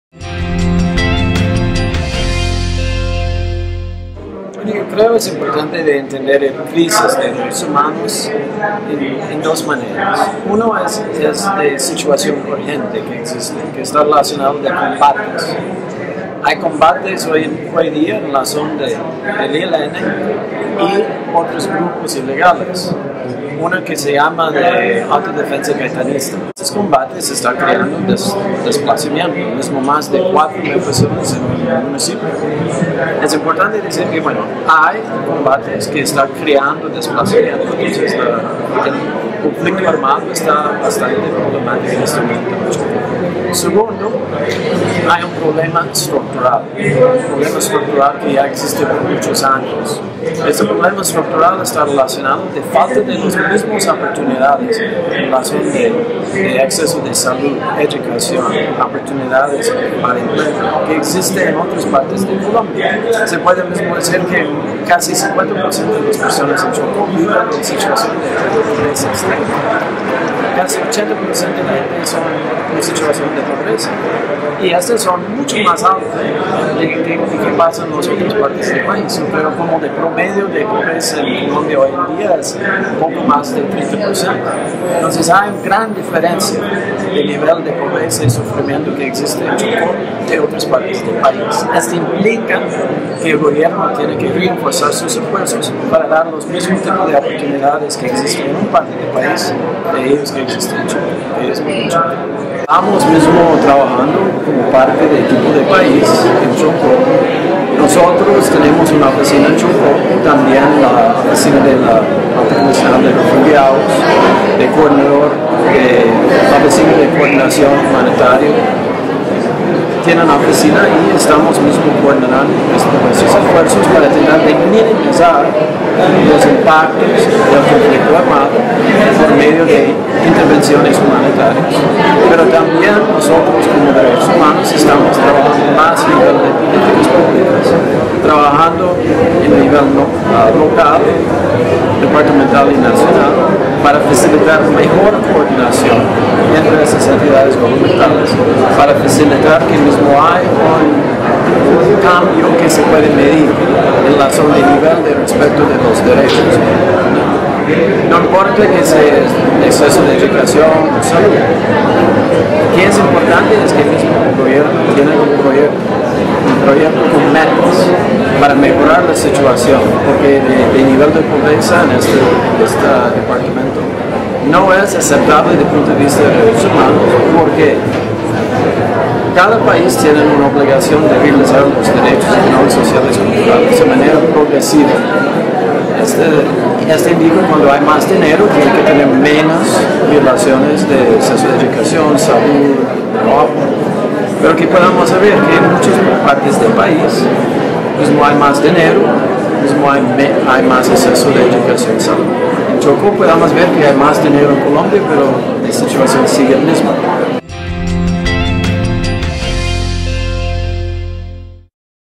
Crisis humanitaria en el Chocó
Todd Howland, representante de la Alta Comisionada de la ONU para los derechos humanos, denuncia el desplazamiento y la difícil situación que se vive en el Chocó.